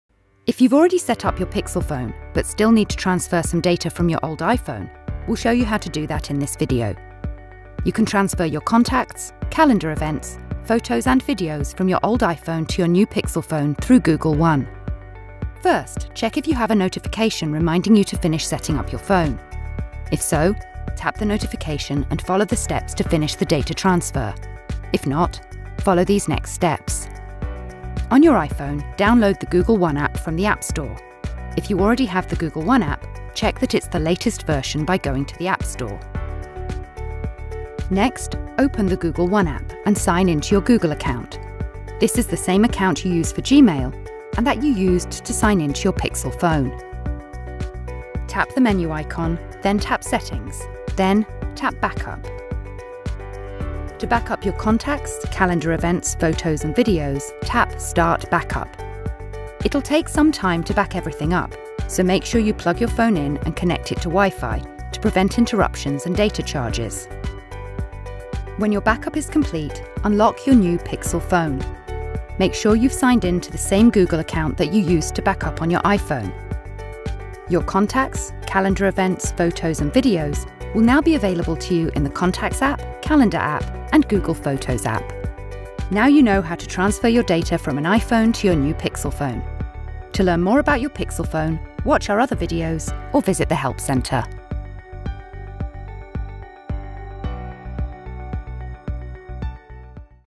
Copy data from your iPhone
Watch this video to learn how to copy data from your iPhone to your Pixel.